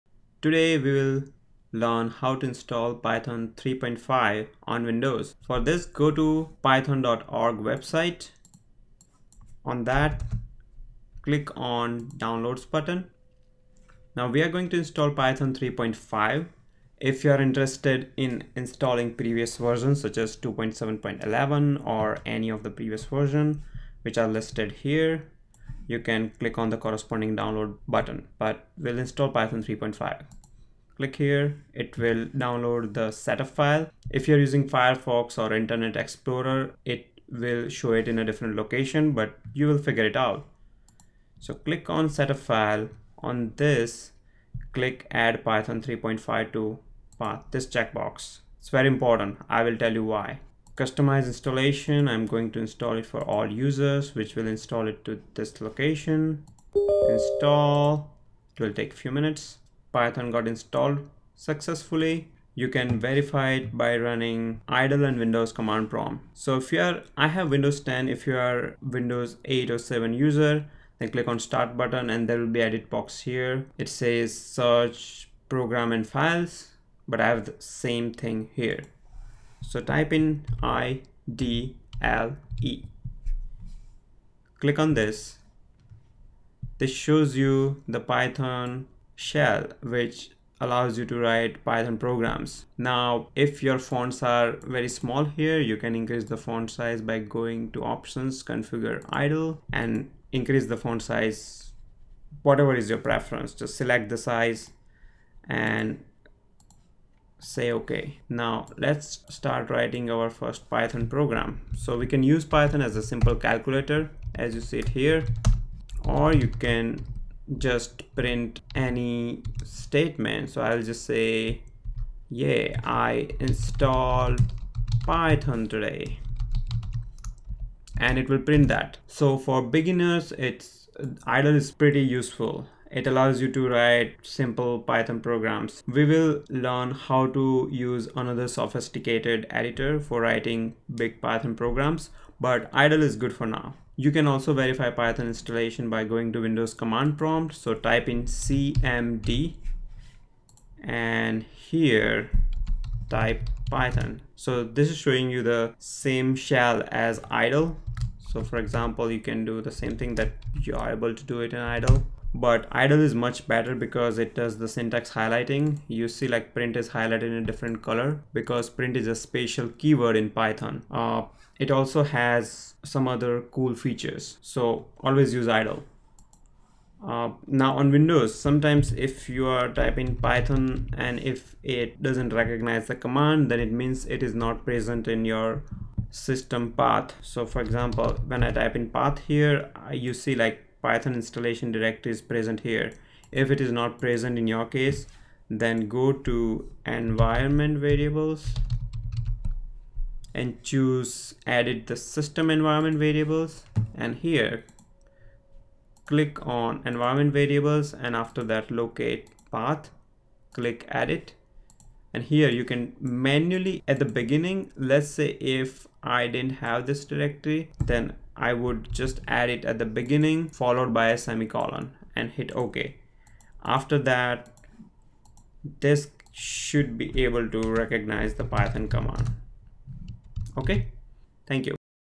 1. Install python on windows [Python 3 Programming Tutorials]
Step by step process on how to install python on windows.

Need help building software or data analytics and AI solutions? My company can help. Click on the Contact button on that website.